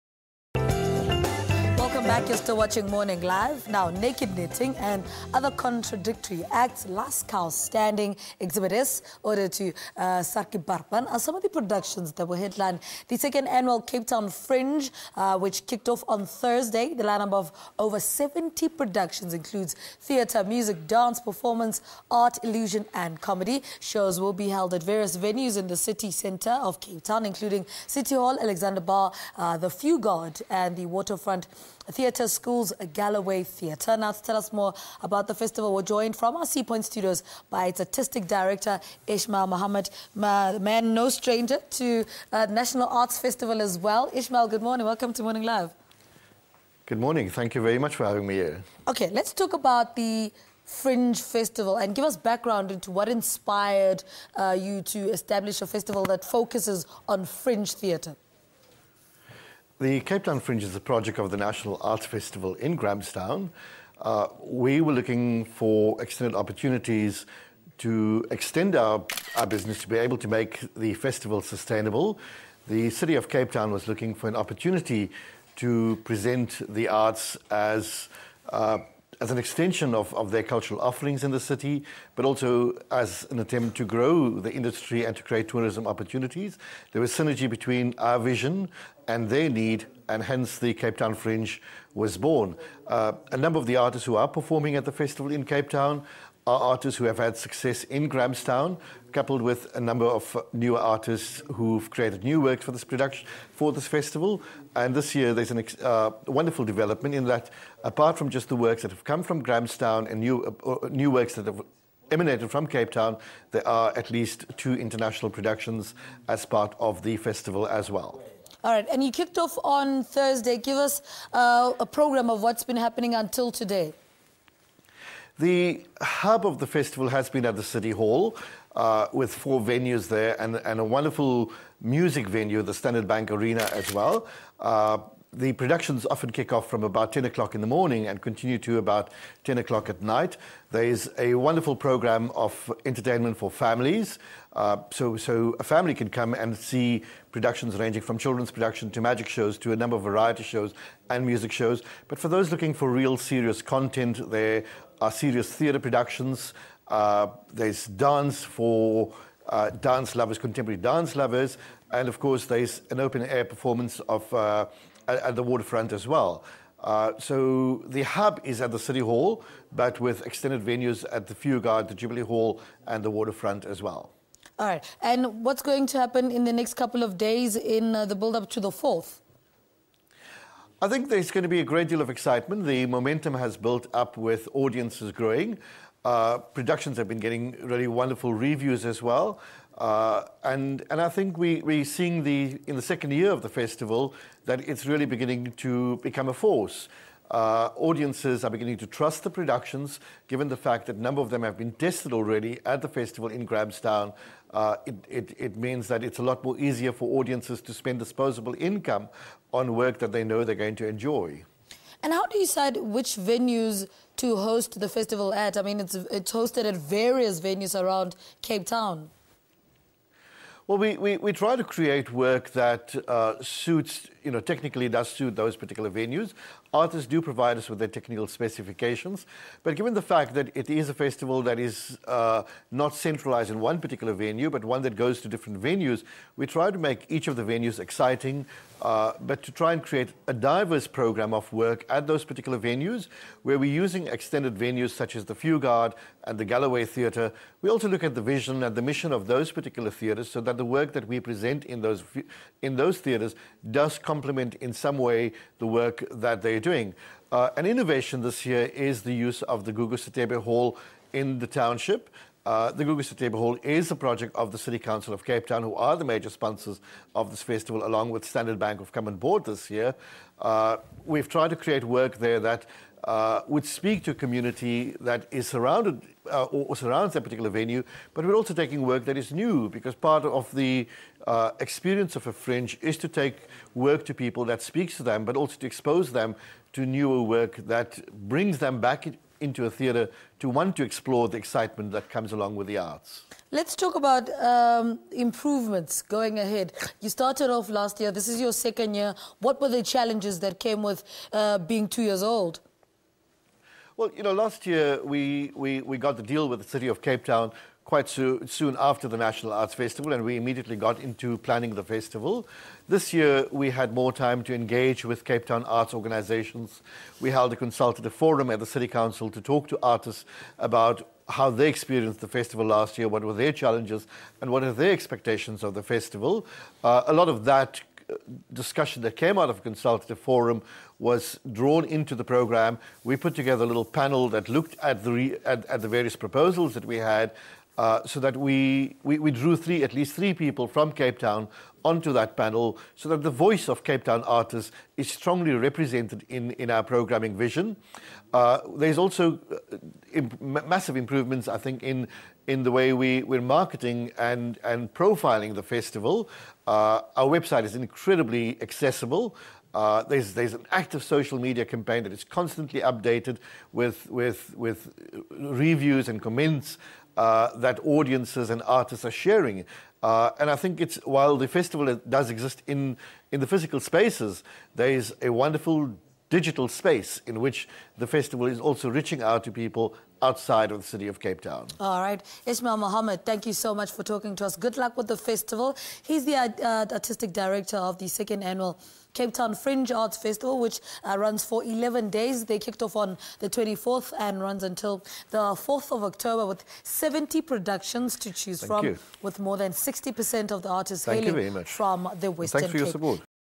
Ismail Mahomed on 2nd Cape Town Fringe Festival
Naked Knitting & Other Contradictory Acts, Last Cow Standing Exhibit S, Ode to Saartjie Baartman are some of the productions that will headline the second annual Cape Town Fringe which kicked off on Thursday. The line-up of 70 productions includes theatre, music, dance, performance art, illusion and comedy. Shows will be held at various venues in the city centre including City Hall, Alexander Bar, the Fugard and the Waterfront Theatre School's Galloway Theatre.